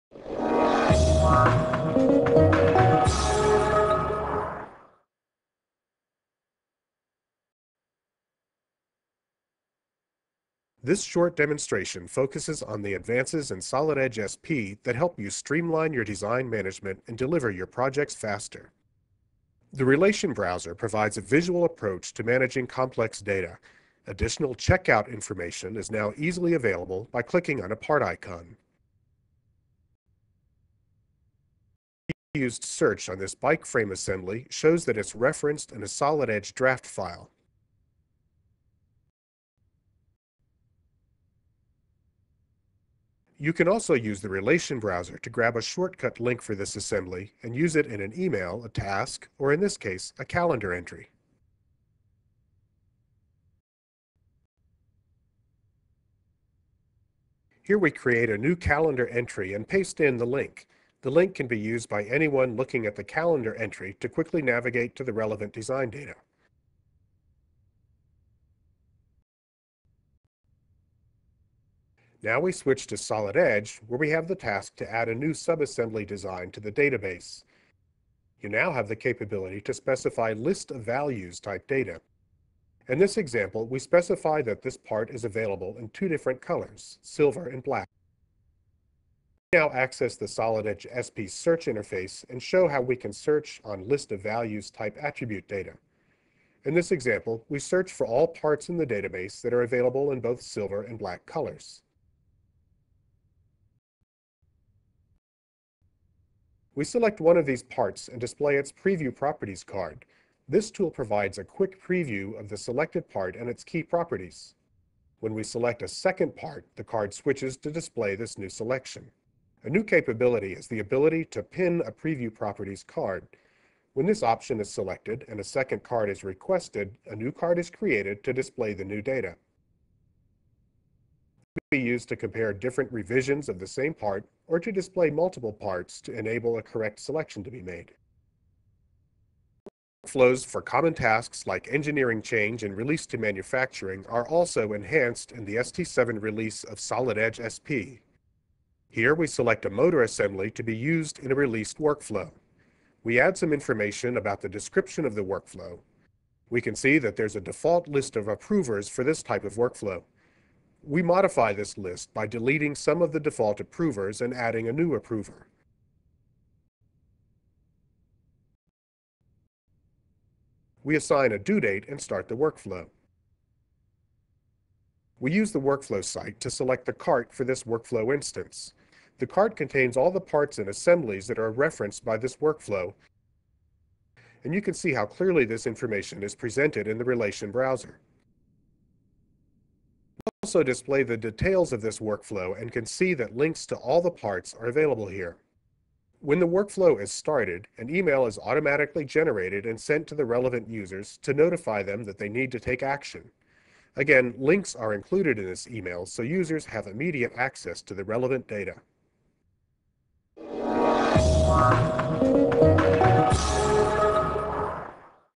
Solid Edge ST7 SP
Solid Edge ST7 SP ile gelen yenilikler için bu videoyu inceleyebilirsiniz.